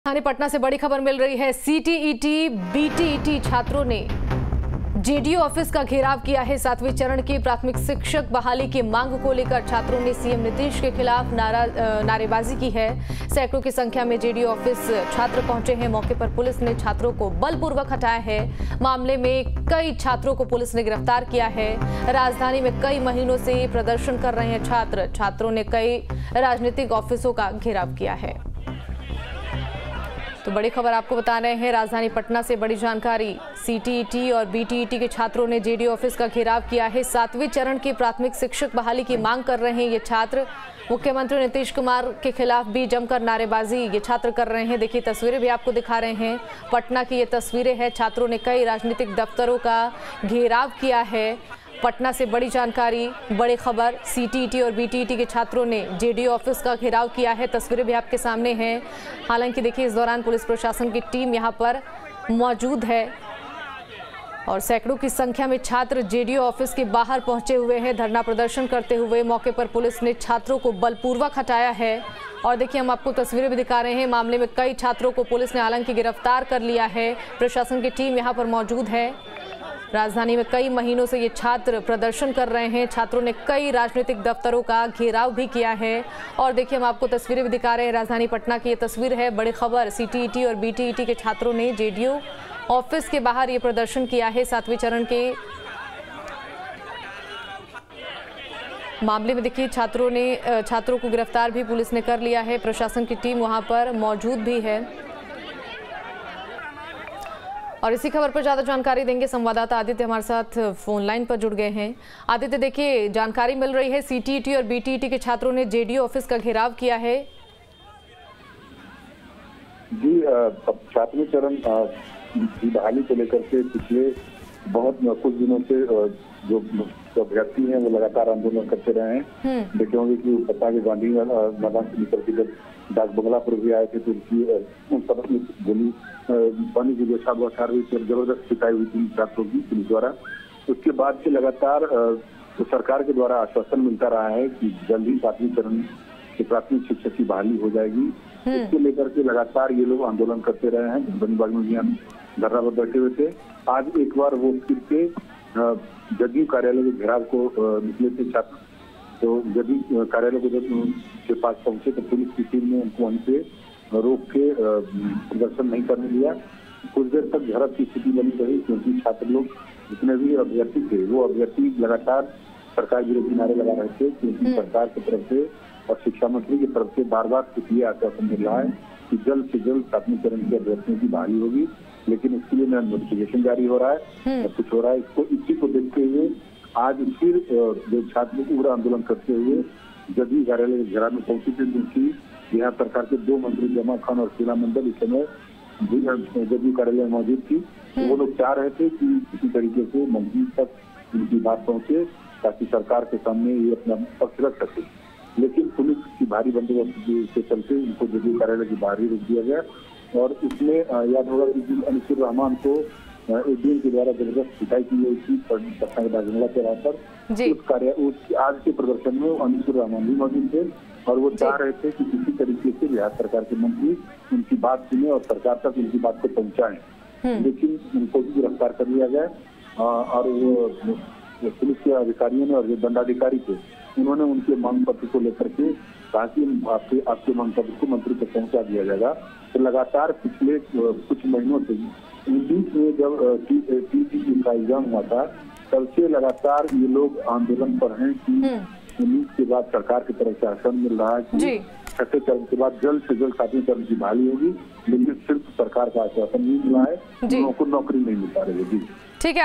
7वें चरण के प्राथमिक शिक्षक बहाली की मांग, नीतीश के खिलाफ की नारेबाजी | Shikshak Bahali | CTET BTET
7वें चरण के प्राथमिक शिक्षक बहाली की मांग, नीतीश के खिलाफ की नारेबाजी | Shikshak Bahali | CTET BTET